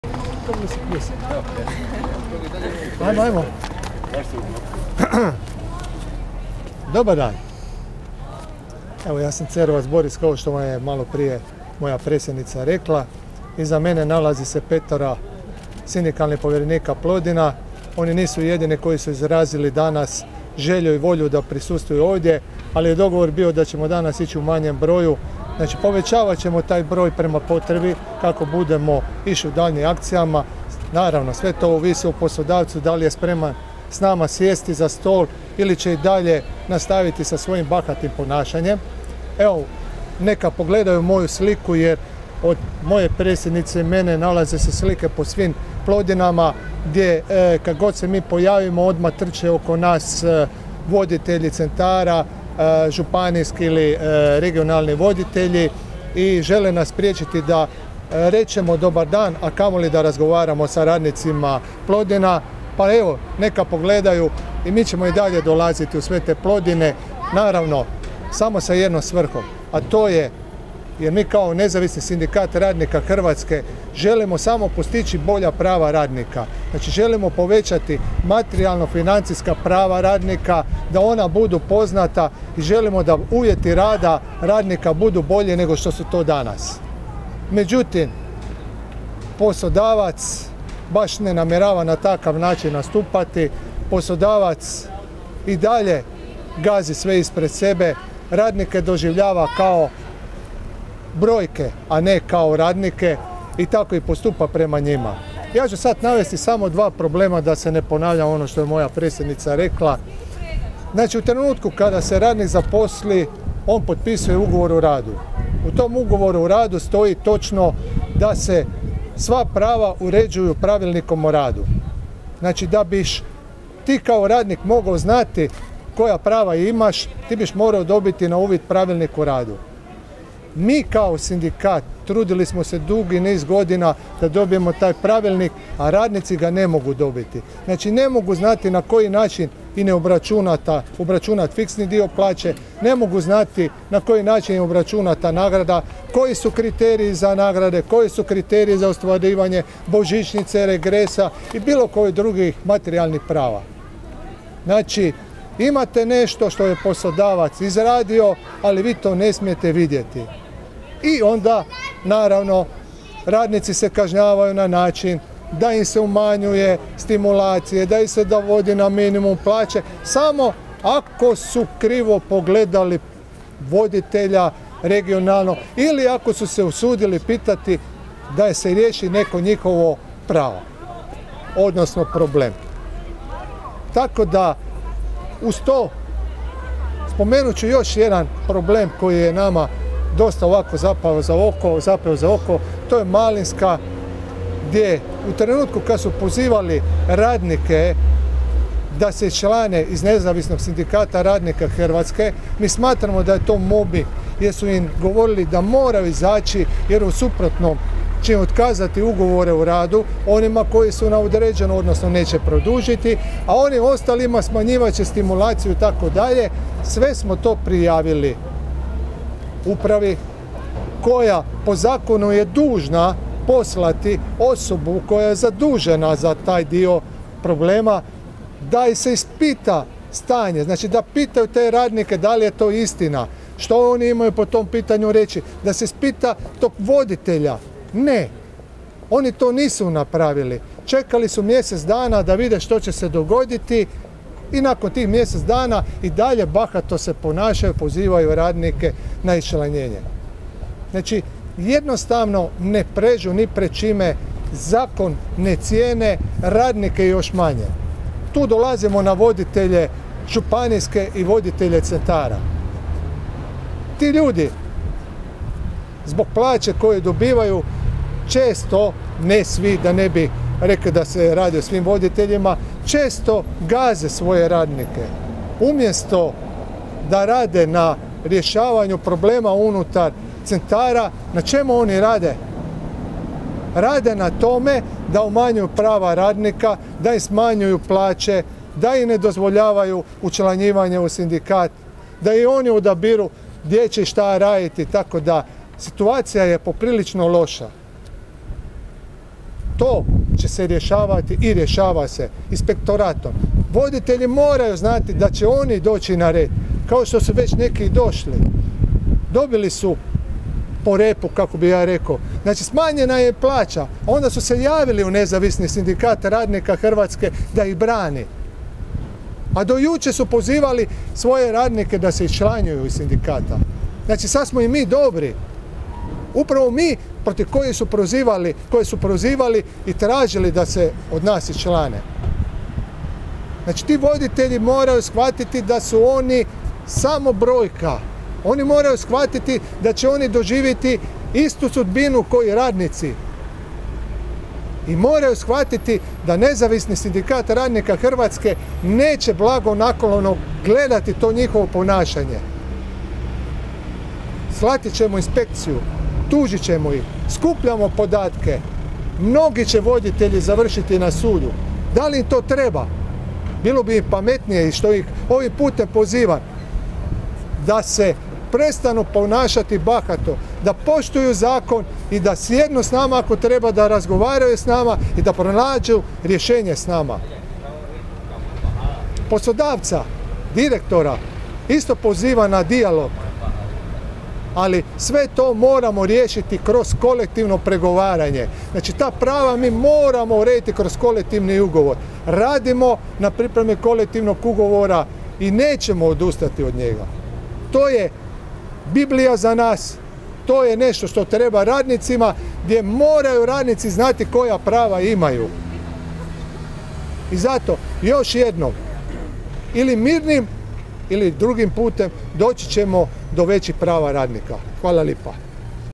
Cerovac o Stanju u Plodinama: "Poslodavac i dalje gazi sve ispred sebe"
Na Trgu svetog Marka održana je konferencija za medije Nezavisnog sindikata radnika Hrvatske na temu grubog kršenja radničkih prava u poduzeću Plodine d.d. iz Rijeke.